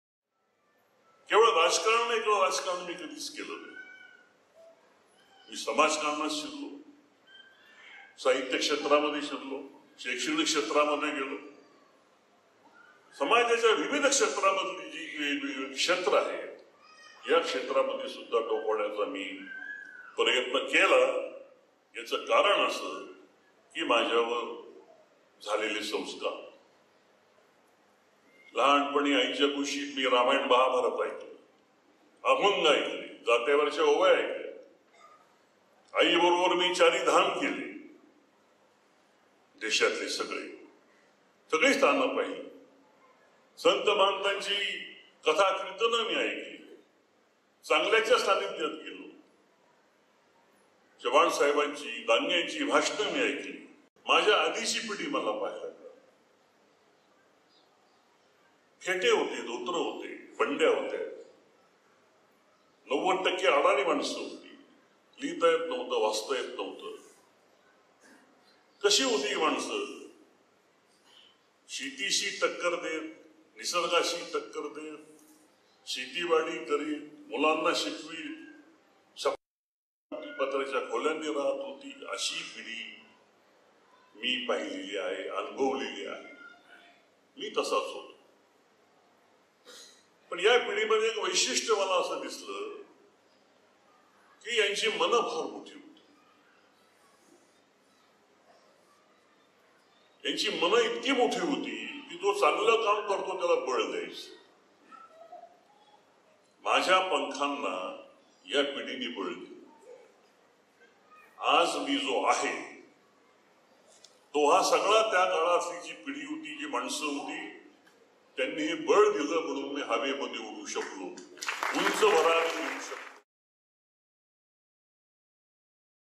Yashwantrao Gadakh
श्री शनैश्वर देवस्थान ट्रस्ट शानि शिंगणापूर यांचे वतीने दिला जाणारा शानिरात्न पुरस्कार प्रदान कार्यक्रम प्रसंगी आपले मनोगत व्यक्त करताना मा.खा.तथा जेष्ठ साहित्यिक यशवंतरावजी गडाख साहेब .दि.६.८.२०१४